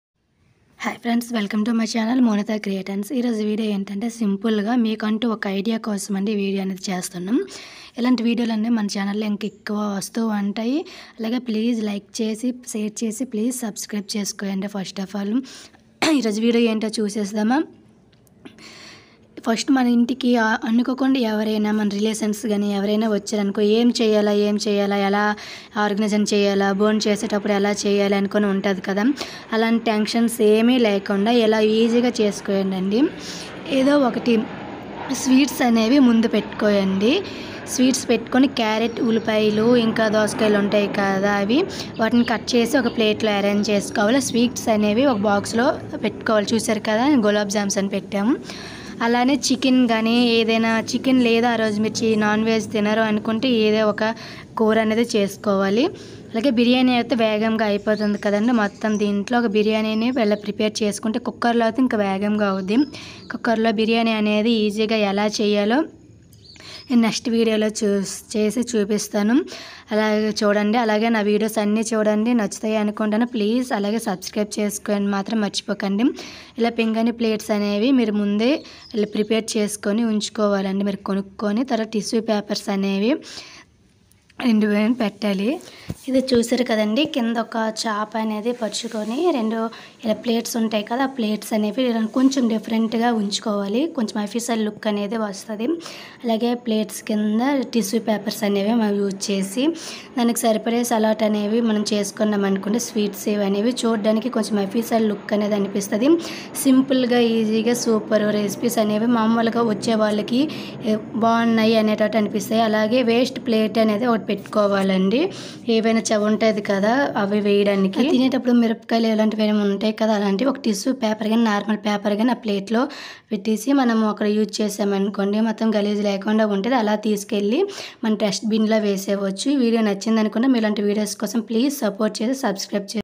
మీ ఇంటికి అనుకోకుండ ఎవరు అయినా సడనగా గెస్ట్ guest వచ్చినప్పుడు ఇలా భోజనం ఏరైంజ్ చేసుకోండి lunch idea
lunch organization ideas